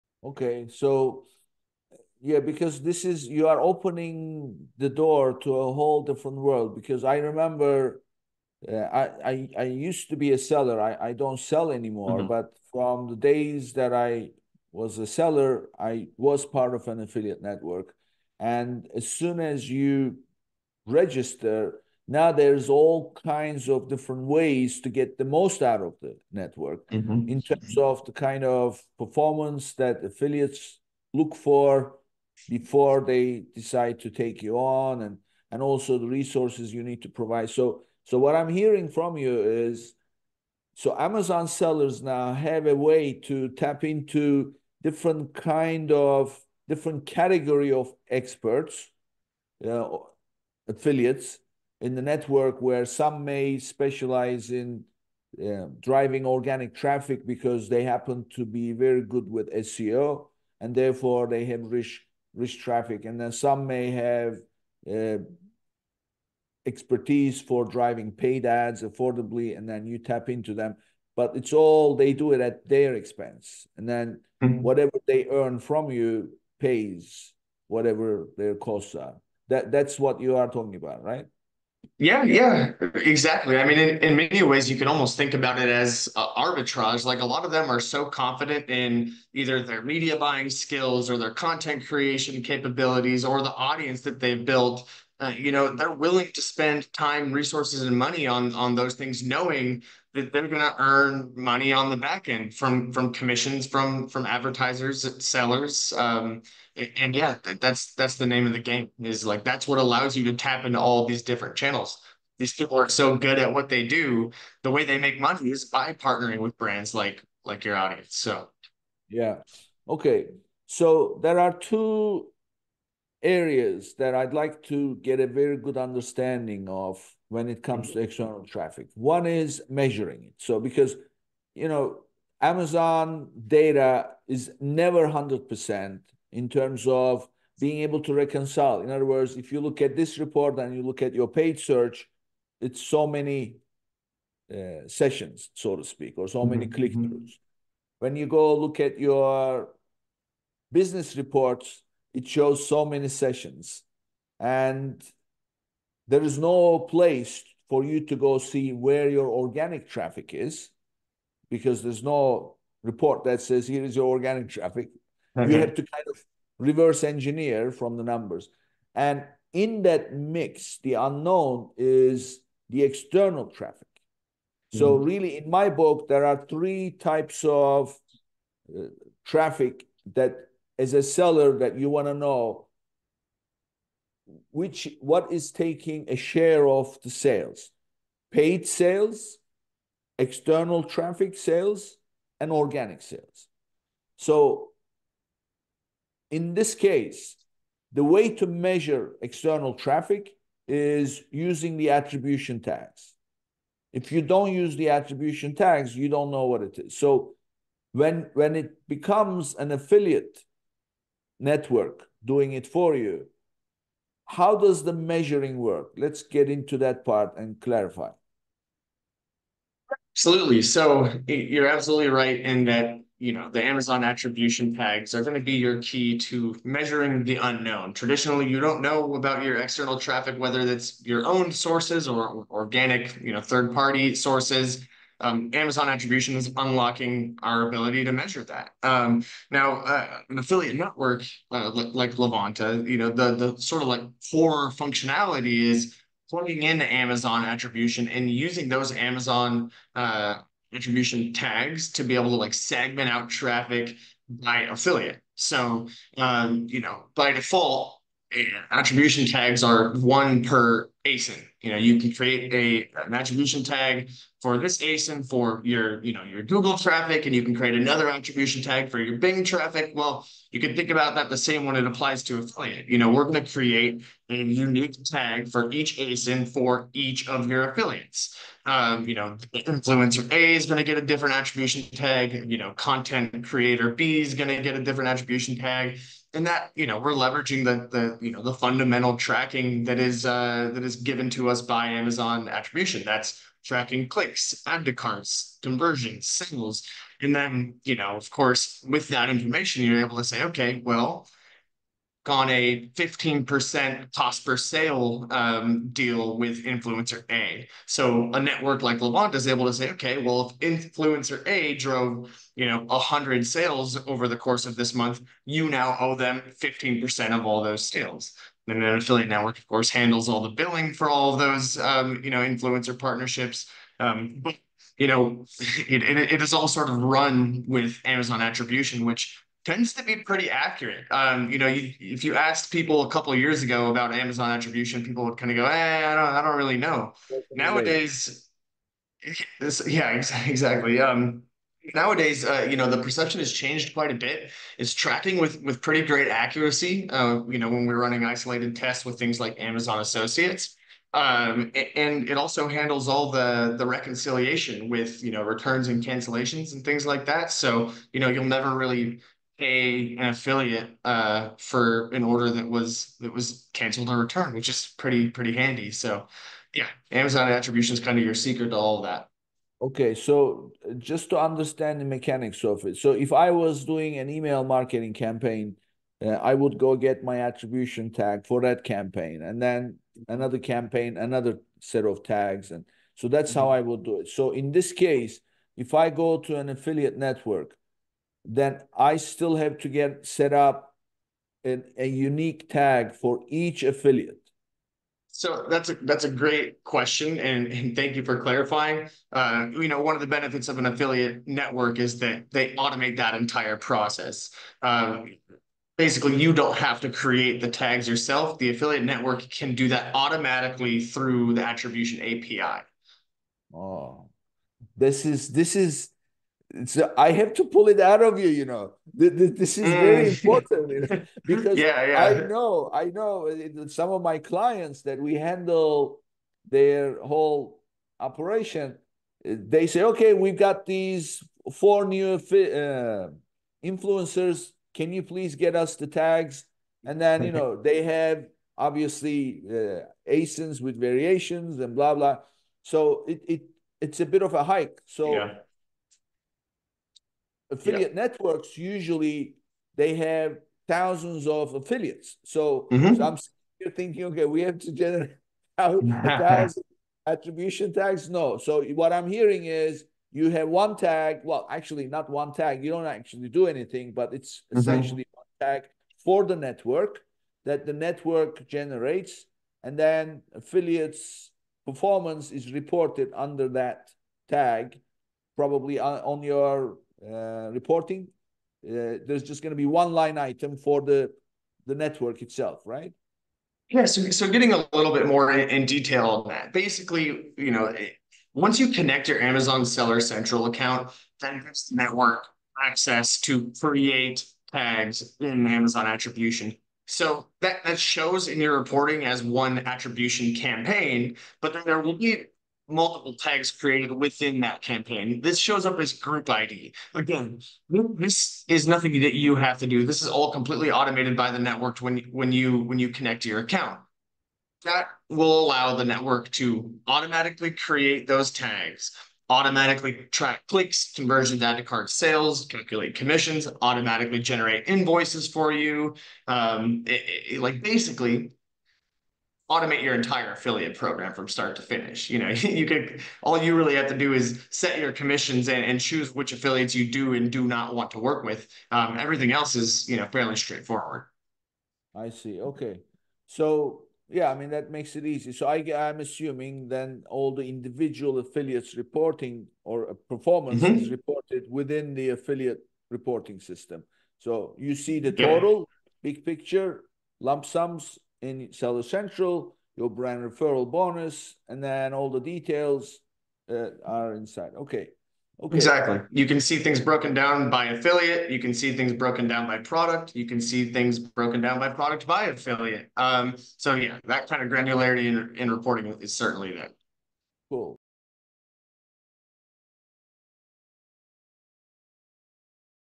Revolutionize Your Amazon Sales with Affiliate Marketing
Welcome to the Amazon Legends Podcast! Discover how to revolutionize your Amazon sales by leveraging affiliate marketing.
In this episode, we explore the power of Amazon attribution, how to work with top affiliates, and the benefits of automated tracking and reporting. Learn why a network of experts can take your sales strategy to the next level.

Don't miss these valuable insights!